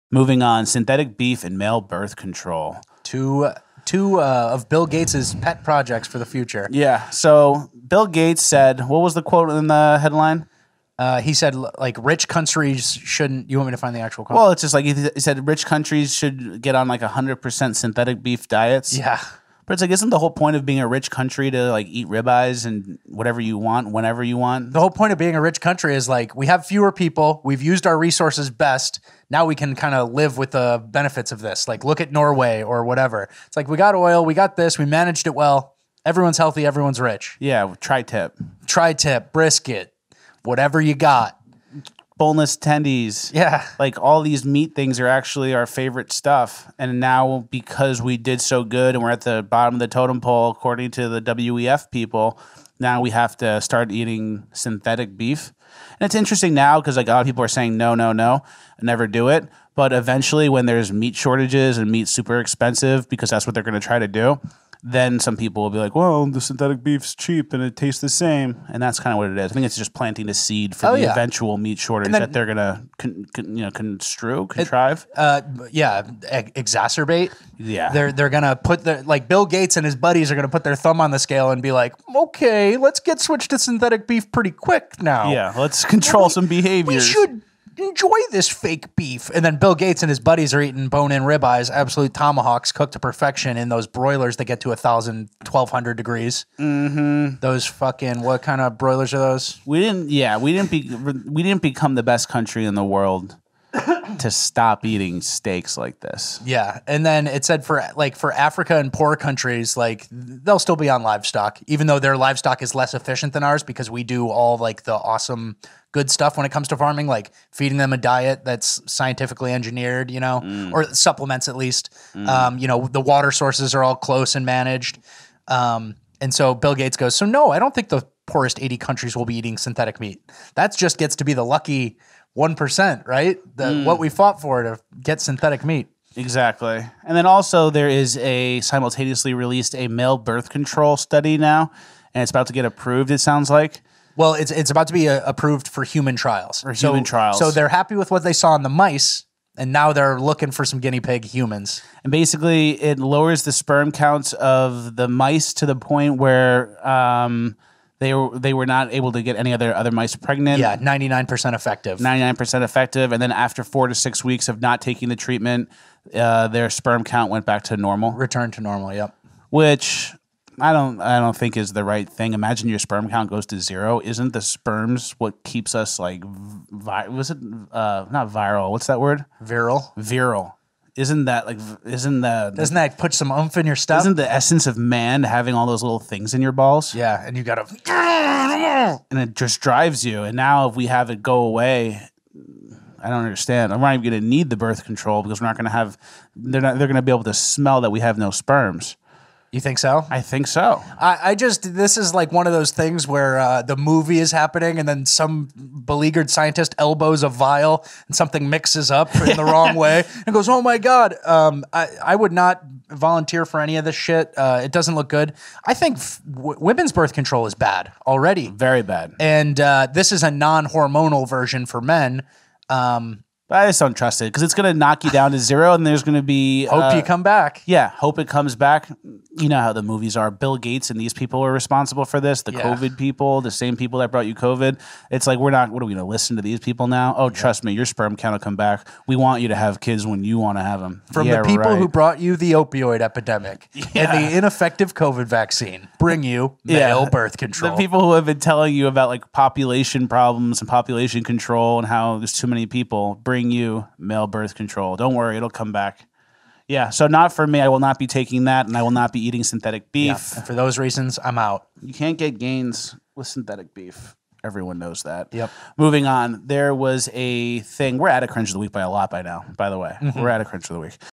Synthetic beef and male birth control? PASS
Thats a no from me dog. Why? Because I'm pretty sure the bad guys are trying to kill us at this point. No need to get involved with this stuff...